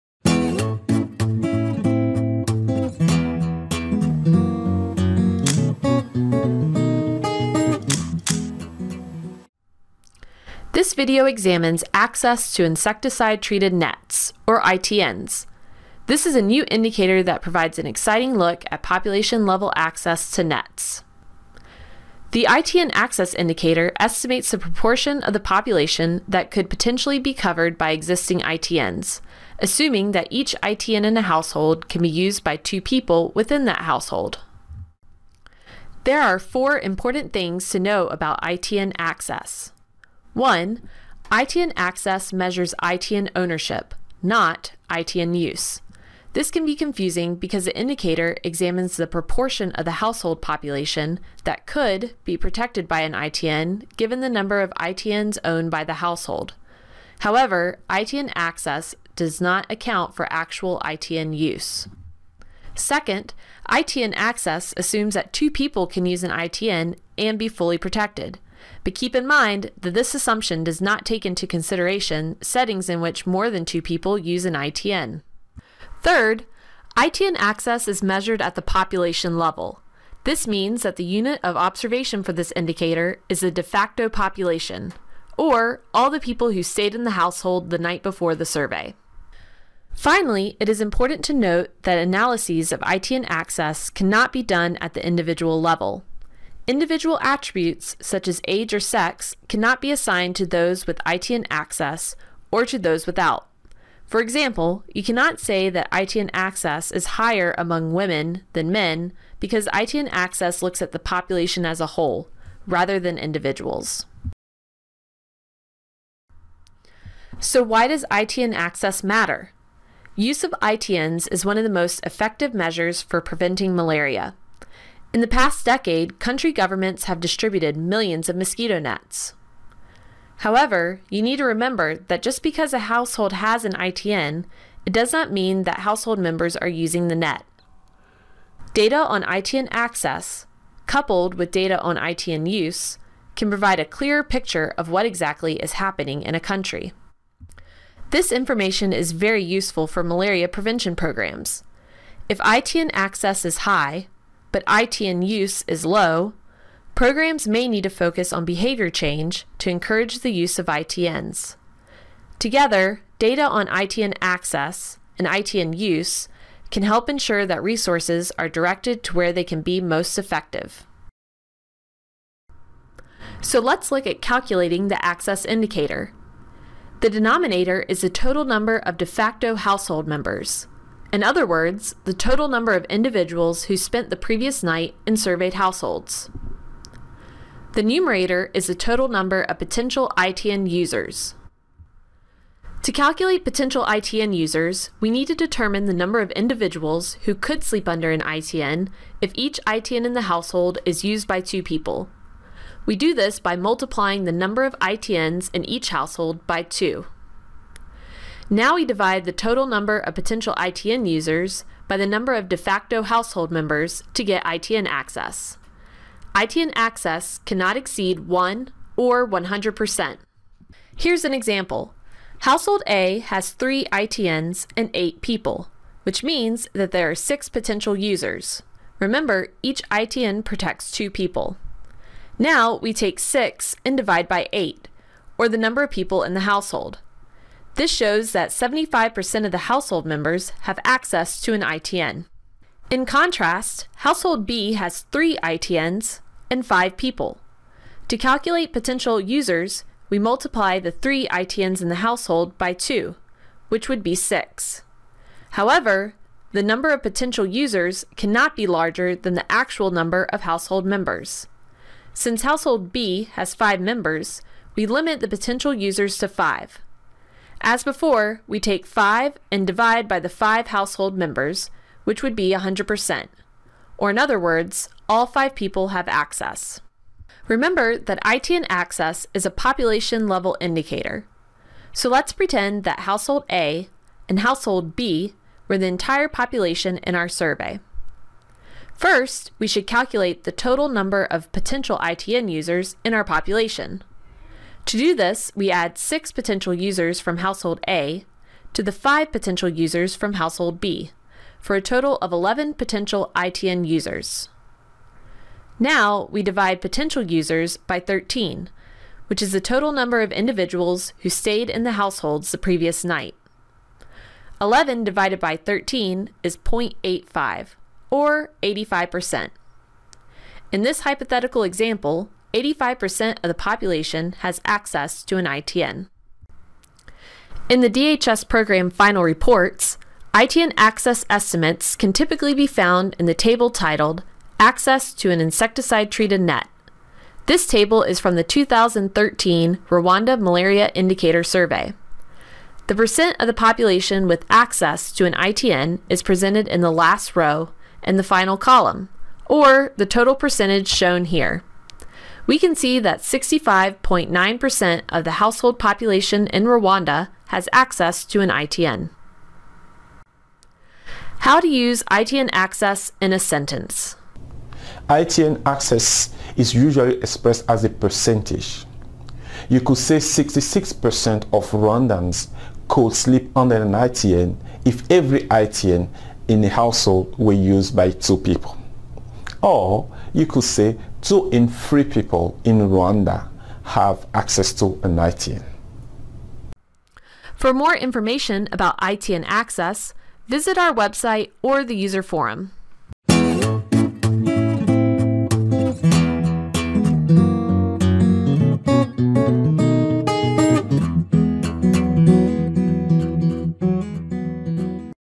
ITN Access Indicator Snapshot
This video provides a explanation of insecticide-treated net (ITN) access including definition, important things to know, why ITN access matters, calculation, where to find it in DHS reports, and how to use it in a sentence.